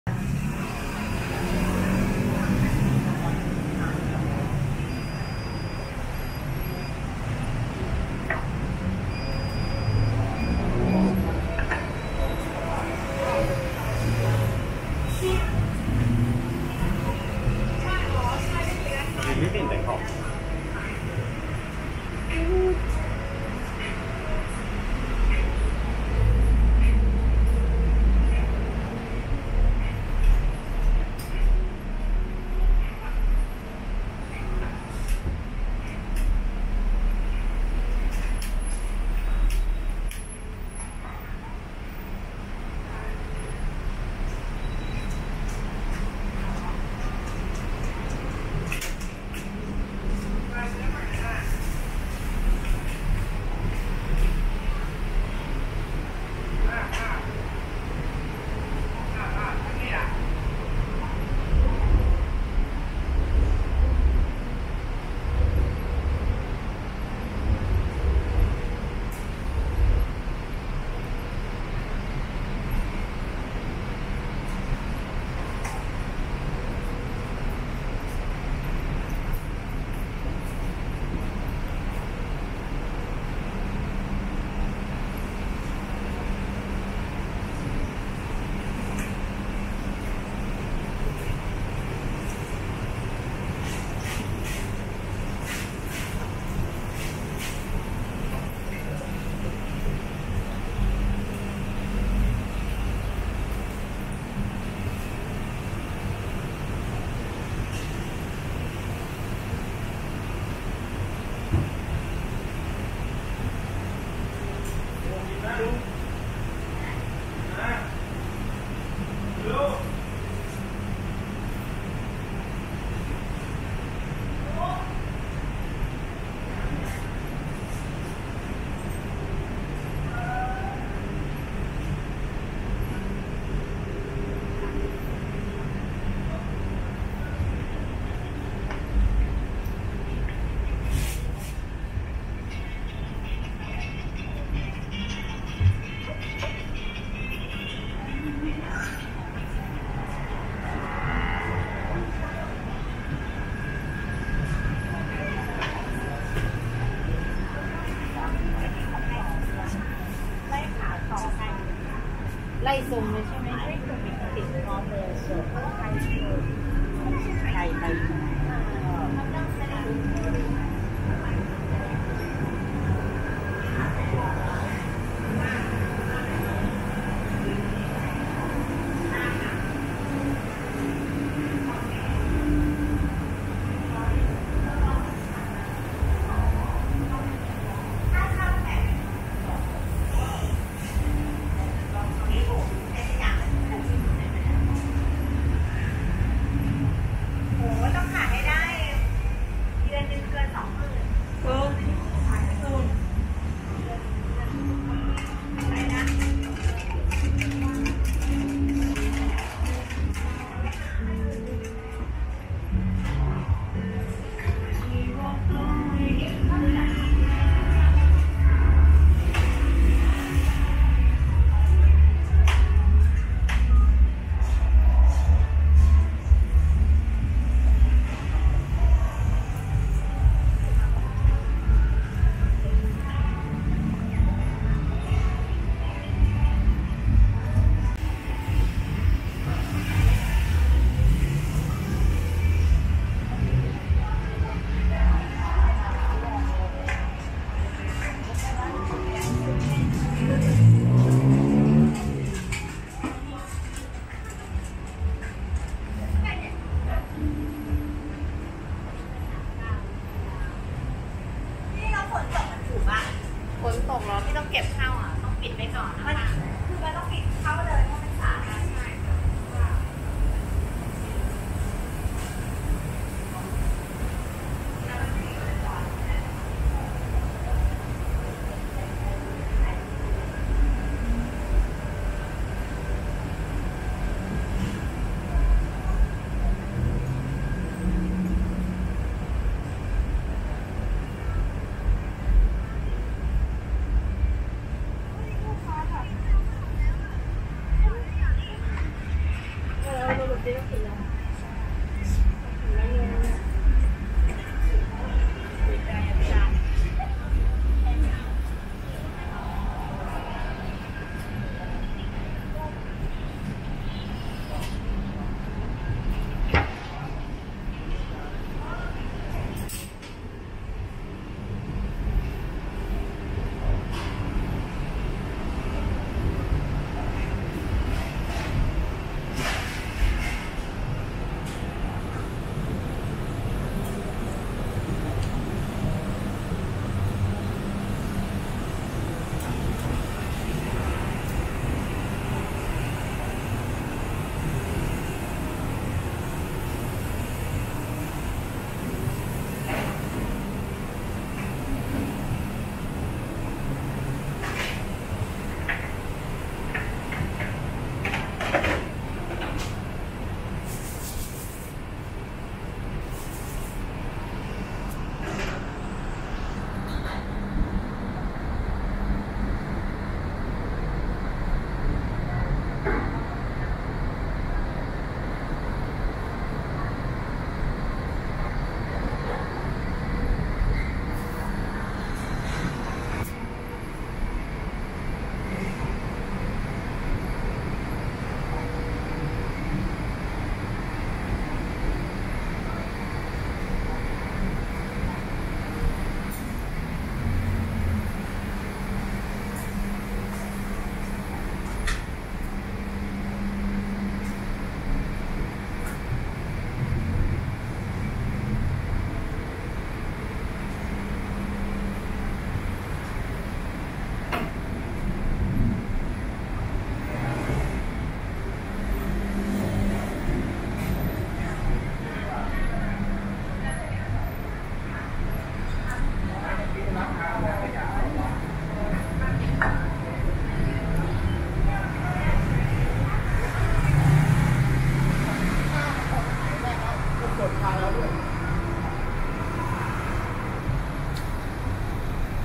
Walk Though Secret Alley 'Siam Soul' In Bangkok
Walk though this unusual secret alleyway of shops in Bangkok. The narrow walkway called Siam Soul is found in the centre of the Thai capital near the Siam district. The area used by street food sellers was closed around two years ago. But it has since re-opened as funky row of small fashion stands selling goods from Japan and Korea. There's even a bizarre foam sculpture protruding from one of the walls. Some shops sell sushi while others have k-pop souvenirs and accessories.